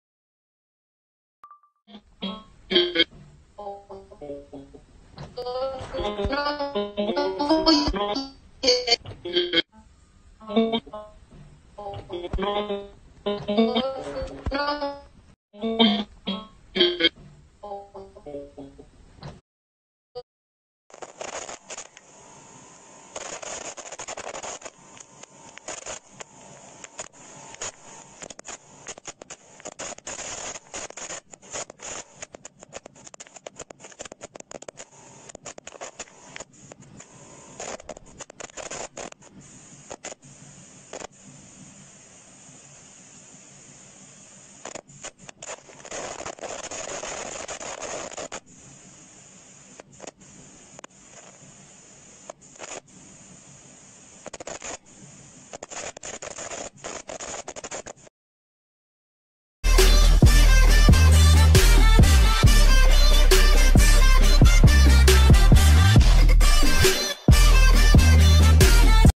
“Network issue” voices to help you escape online classes || Life hacks
Don’t know the answer to a question? We got you covered.
Use these voices into your computer’s microphone and you’ll get away with it.